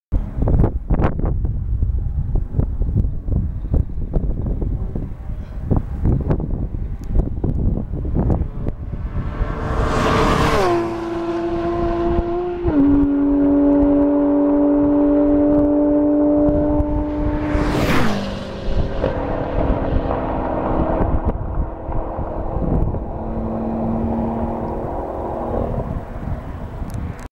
911 GT3 and BMW M5 Fly BY
A 2010 911 GT3 and a 2013 M5 fly-by at Brainerd Motor Speedway during the 2012 OLOA event. Speeds at the end of the straight are around 165mph. Drivers Carl Stringer, Mike Renner.